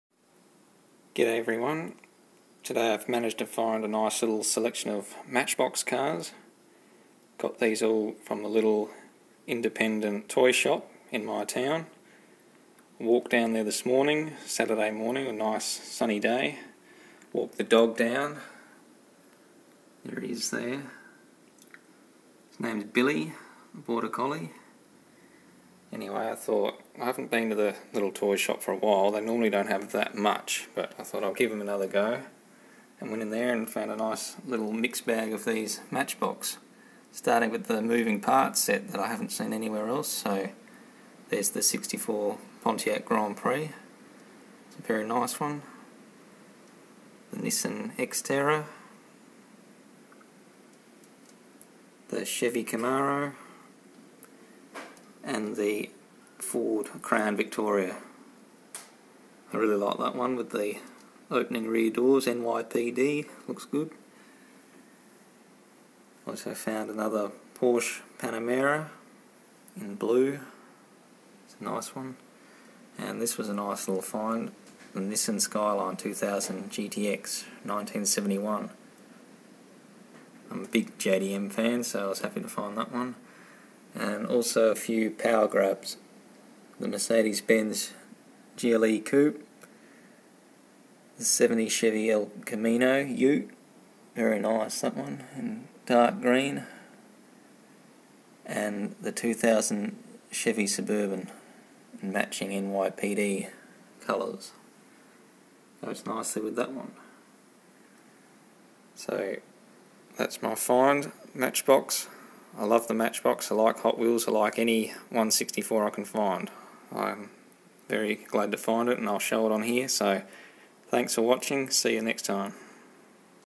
Matchbox Moving Parts, Power Grabs & More
Nice Matchbox Finds at the local Toy Shop